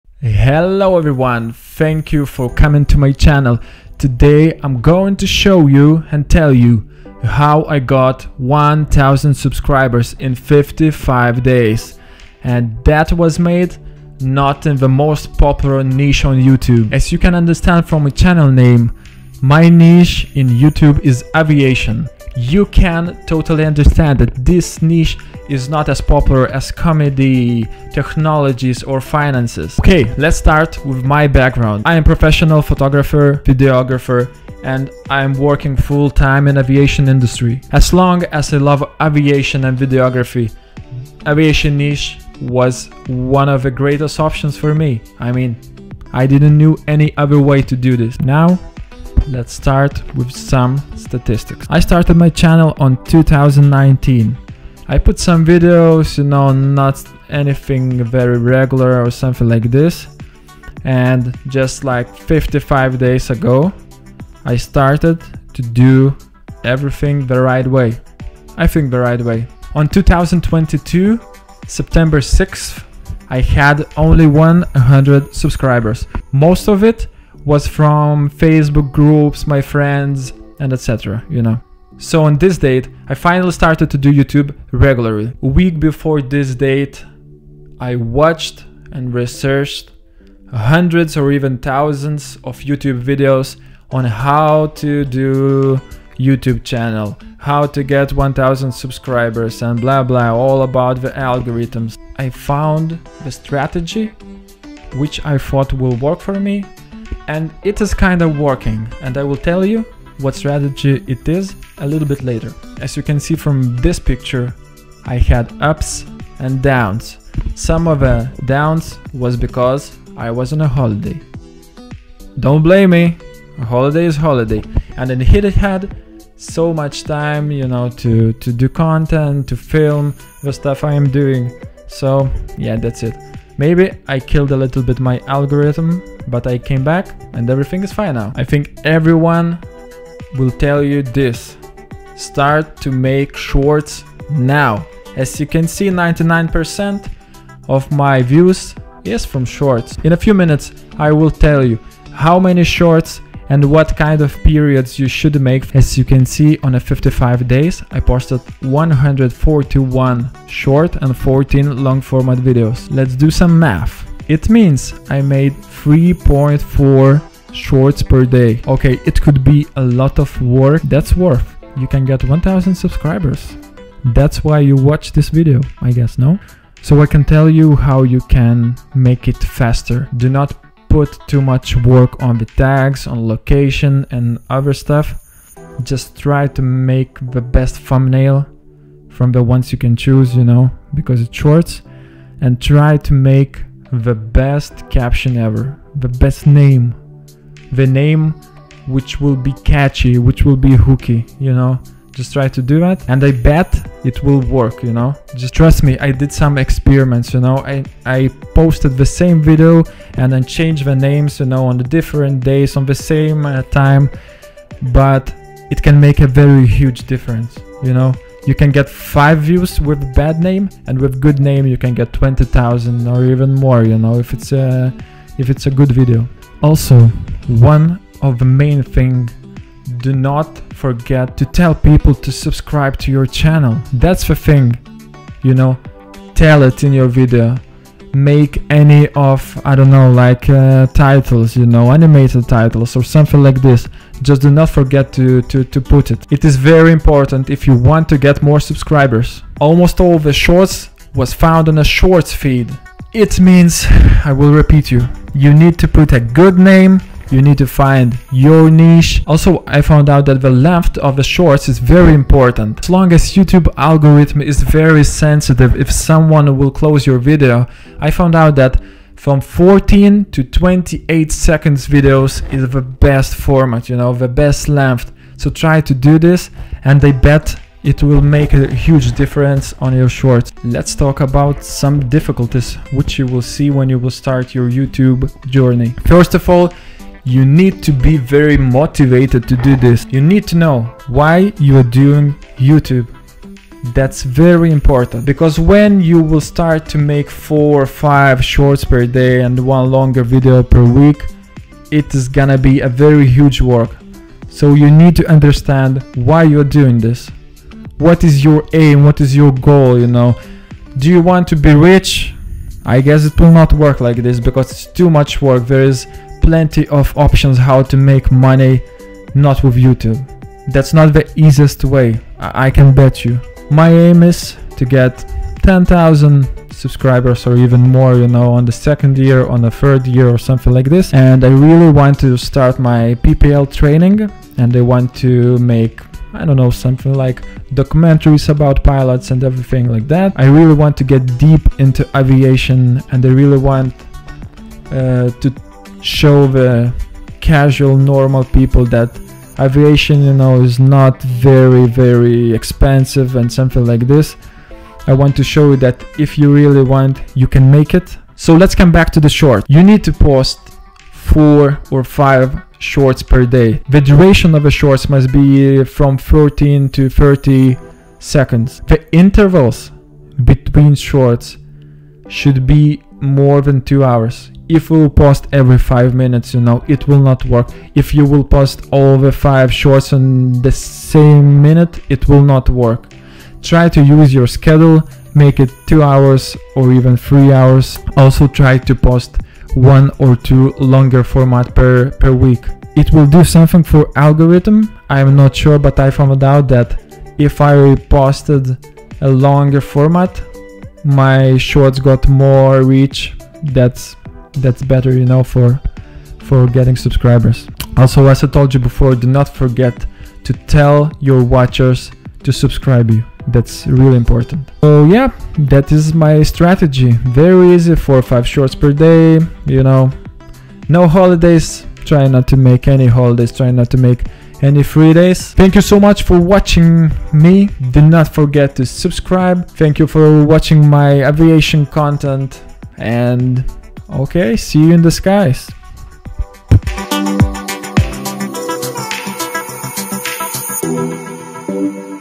HOW TO GET 1000 SUBSCRIBERS IN 2023 (FOR FREE)
In this video, we're going to show you how to get 1000 subscribers on YouTube in just 2023 (for free)!

If you're looking to build a successful youtube channel, then you need to start by getting 1000 subscribers as quickly as possible. This is a relatively easy task, and we'll show you how to do it in just a few easy steps. Check out this video to learn more about how to get 1000 subscribers on YouTube for free!
Hello people. On this video I will tell you how I got my 1000 subscribers in 7 weeks or 55days. It wasn't so bad ! Nothing very crazy, just a little strategy and that's it. Watch till the end to see all the tricks.